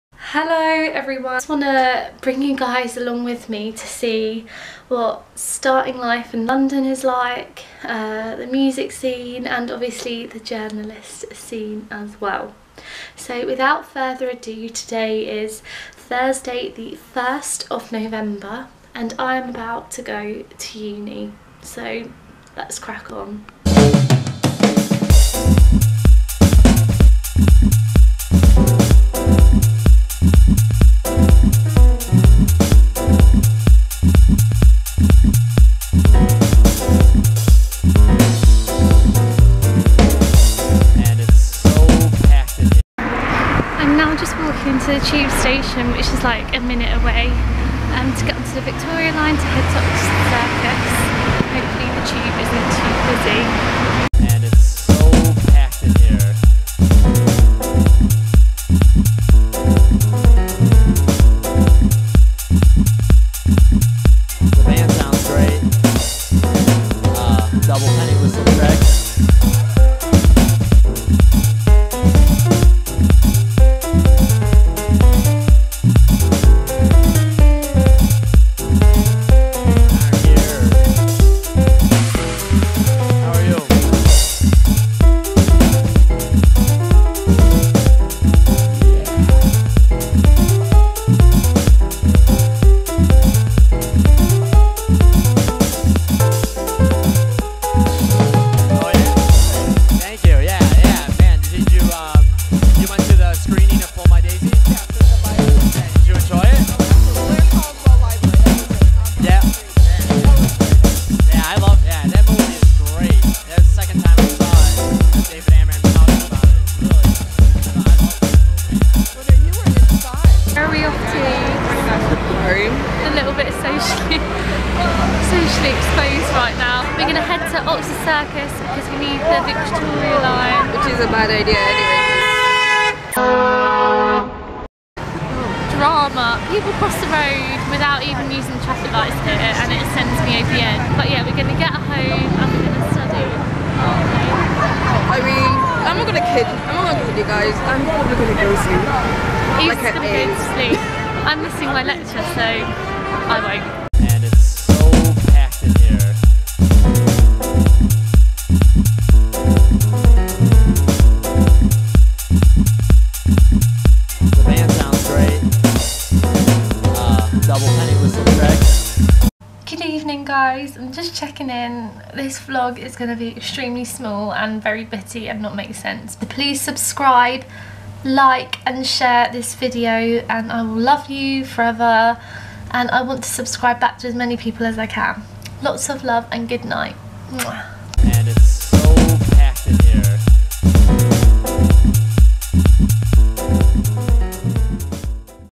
THE START OF NOVEMBER AND JAYWALKING | VLOG | SCARLETT NOTES
Well it has been a hot minute beauties!

Here is just a little UNI day VLOG. Saying a little hey to my gal, Eva who is featuring in it.

Take it easy x

SOCIAL:

Blog: Scarlett Notes Blog